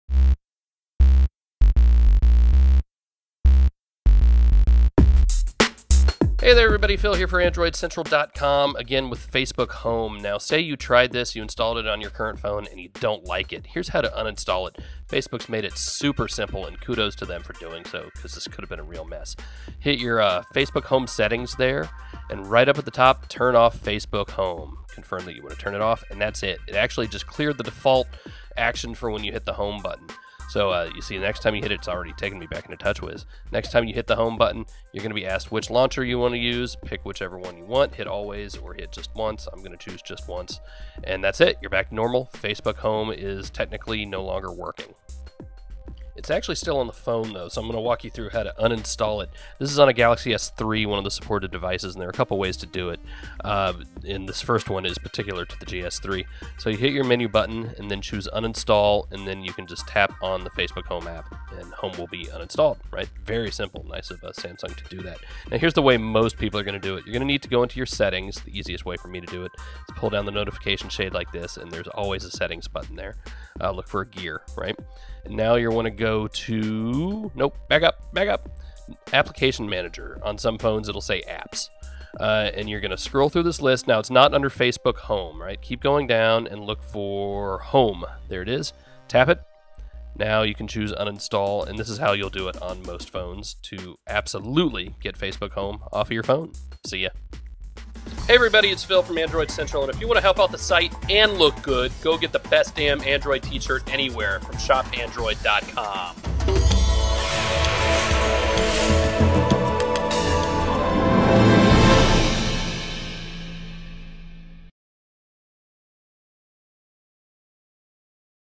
How to uninstall Facebook Home from your Android smartphone
We walk through how to remove and uninstall Facebook Home from your current Android smartphone. At launch, Facebook Home is available on the HTC One X, HTC One X+, Samsung Galaxy S3 remove Galaxy Note 2.